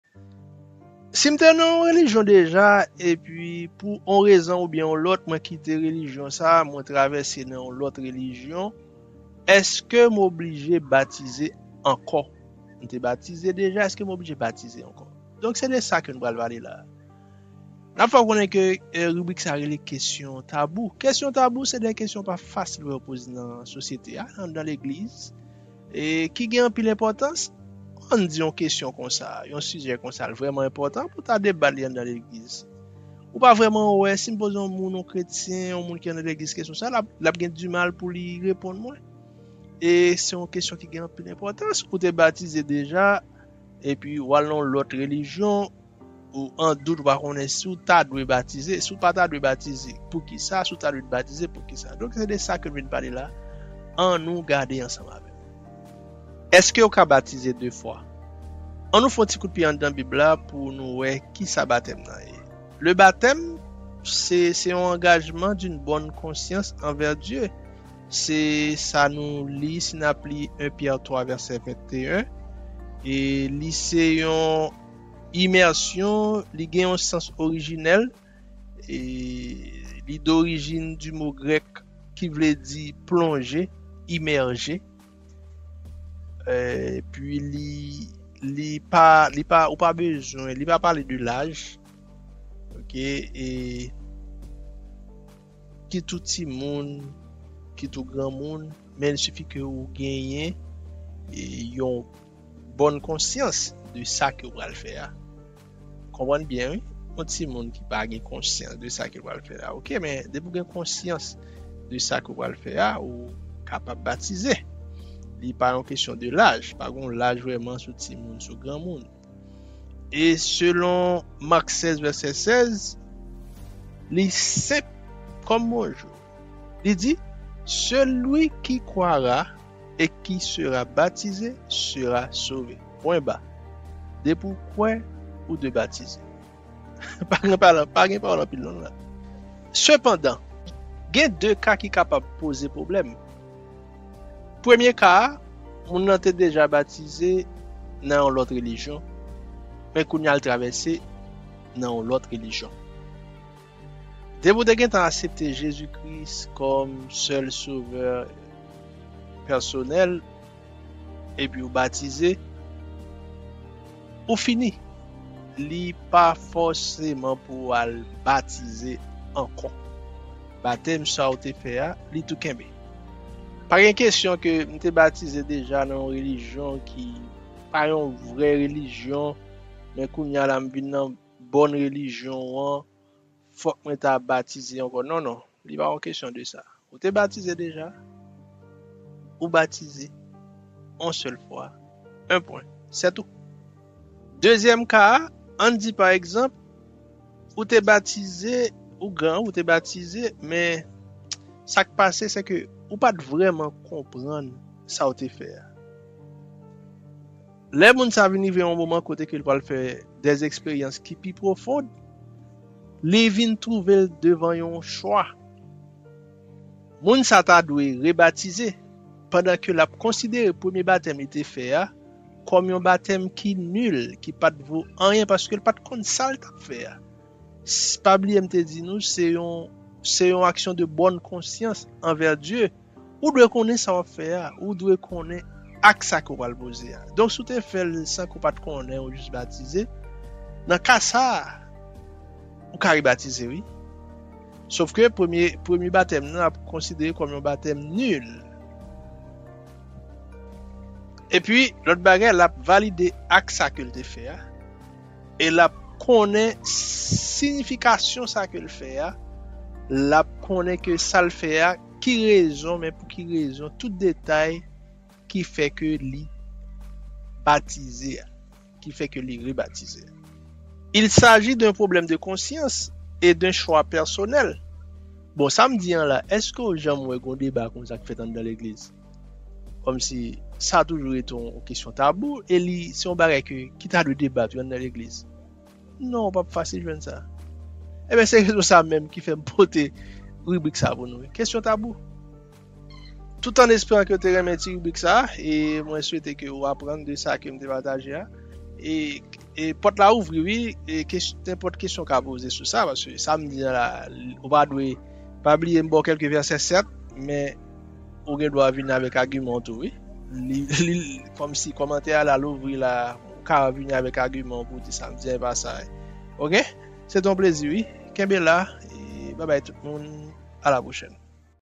Eske w kapab batize 2 fwa?Peut on se faire baptiser 2 fois?
Ann gad video sa pou nou konnen si sa posib